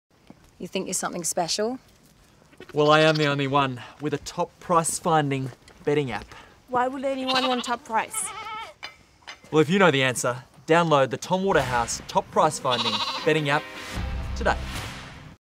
Tom Waterhouse tells you how to ensure you're getting the Best Prices betting
Getting an advantage over the Bookies isn't easy but using the Tom Waterhouse App ensure's you have the advantage against the Bookies getting the Top Price in the Market and put the Odds in your favour.